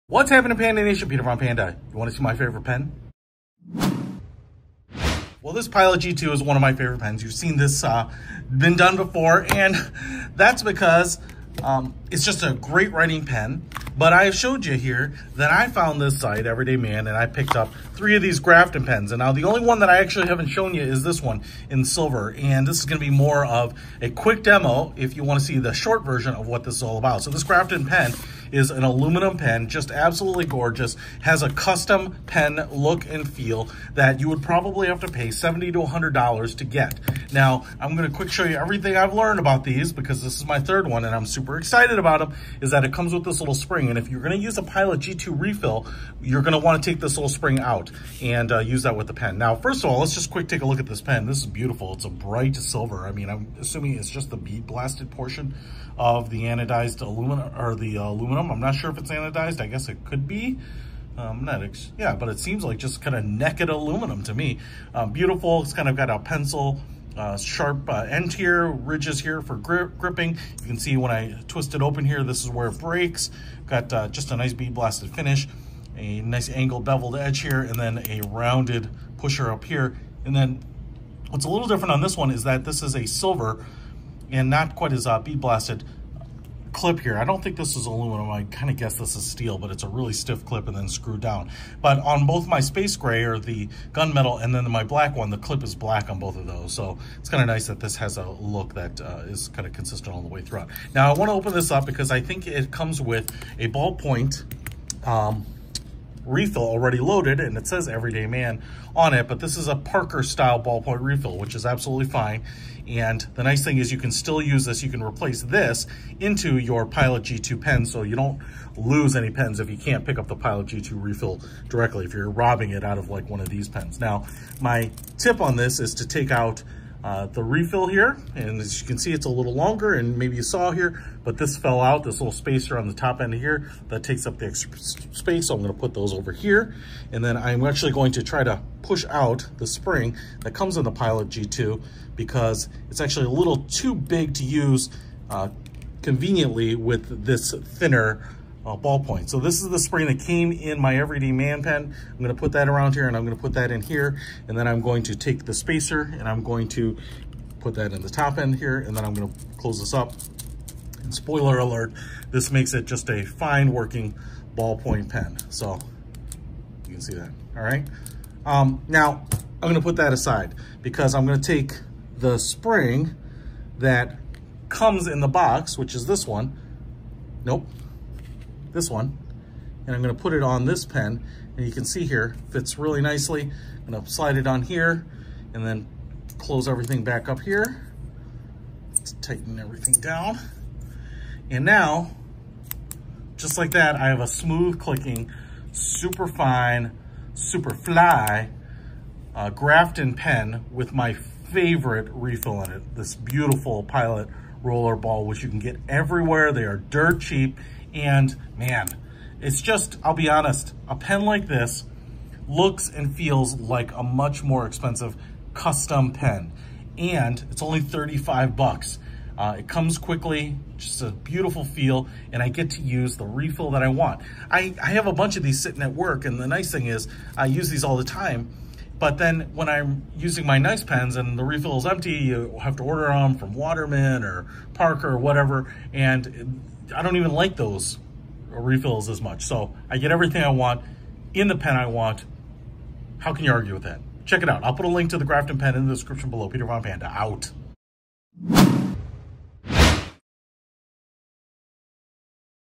The Everyman Grafton Aluminum Pen Works With Different Types of Refills
GRAFTON PEN

The thoughtfully designed, buy-for-life pen that won't break the bank. Grafton will also take Bic Gel, Uniball Jetstream, Pilot G2, Fisher Space Pen, Monteverde Ballpoint, and both Parker Gel and Ballpoint refill cartridges.

For use with the new & improved Grafton refills as well as Fisher Space Pen Ballpoint, Monteverde Ballpoint, and both Parker Gel and Ballpoint refill cartridges use the spacer and spring that comes inside the pen. For use with Bic Gel, Uniball Jetstream and Pilot G2 use spring that you'll find in the foam/box.

Don't forget extra refills. SPECS

100% Anodized 6061 Aluminum Pen Body
Heavy Duty Bolt On Clip
Expertly Weighted
Reliable Buy-For-Life Design
Deep Groove Grip
Weight: 1oz
Length: 145mm
Diameter: 11mm Expedition Money LLC
A Financial Wellness Program That Works